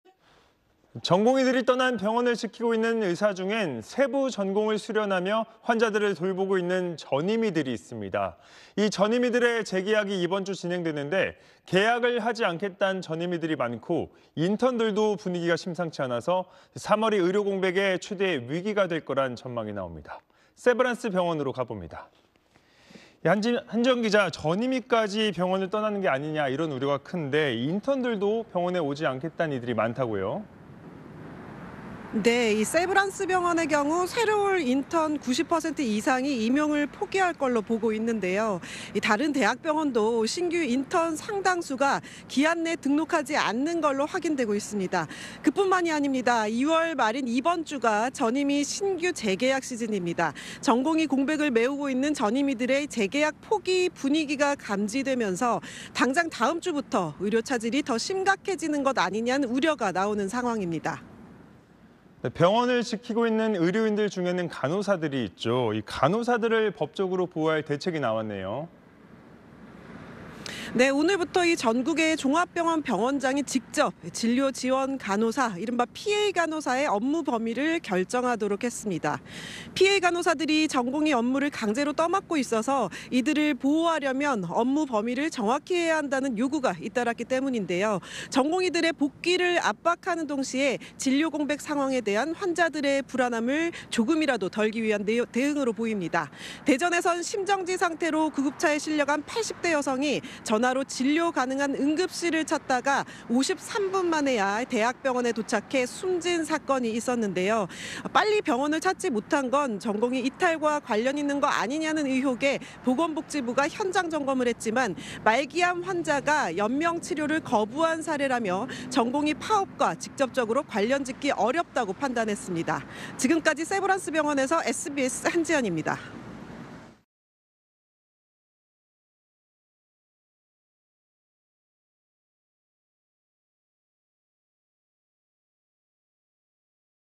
[현장] 전임의도 '이탈' 가시화…'3월 위기' 폭풍전야 / SBS
〈앵커〉

전공의들의 떠난 병원을 지키고 있는 의사 중엔 세부 전공을 수련하며 환자들을 돌보고 있는 전임의들이 있습니다. 이 전임의들의 재계약이 이번주 진행되는데 계약을 하지 않겠단 전임의들이 많고, 인턴들도 분위기가 심상치 않아서 3월이 의료 공백의 최대 위기가 될 거란 전망이 나옵니다. 세브란스 병원으로 가봅니다.

한지연 기자, 전임의까지 병원을 떠나는 게 아니냔 우려가 큰데 인턴들도 병원에 오지 않겠단 이들이 많다고요?

〈기자〉

세브란스병원의 경우 새로 올 인턴 중 90% 이상이 임용을 포기할 걸로 보고 있는데요.

다른 대학병원도 신규 인턴 상당수가 기한 내 등록하지 않는 걸로 확인되고 있습니다.

그뿐만이 아닙니다.

2월 말인 이번 주가 전임의 신규 재계약 시즌입니다.

전공의 공백을 메우고 있는 전임의들의 재계약 포기 분위기가 감지되면서 당장 다음 주부터 의료 차질이 더 심각해지는 것 아니냔 우려가 나오는 상황입니다.

〈앵커〉

병원을 지키고 있는 의료인들 중에는 간호사들이 있죠. 이 간호사들을 법적으로 보호할 대책이 나왔네요?

〈기자〉

오늘(27일)부터 전국의 종합병원 병원장이 직접 진료지원 간호사, 이른바 PA 간호사의 업무 범위를 결정하도록 했습니다.

PA 간호사들이 전공의 업무를 강제로 떠맡고 있어서, 이들을 보호하려면 업무 범위를 정확히 해야 한다는 요구가 잇따랐기 때문인데요.

전공의들의 복귀를 압박하는 동시에 진료공백 상황에 대한 환자들의 불안함을 조금이라도 덜기 위한 대응으로 보입니다.

대전에선 심정지 상태로 구급차에 실려간 80대 여성이 전화로 진료 가능한 응급실을 찾다가 53분 만에야 대학 병원에 도착해 숨진 사건이 있었는데요.

빨리 병원을 찾지 못한 건 전공의 이탈과 관련 있는 거 아니냐는 의혹에 보건복지부가 현장 점검을 했지만, 말기암 환자가 연명치료를 거부한 사례라며 전공의 파업과 직접적으로 관련을 짓기 어렵다고 판단했습니다.

(영상취재 : 양두원, 영상편집 : 김진원, 현장진행 : 편찬형)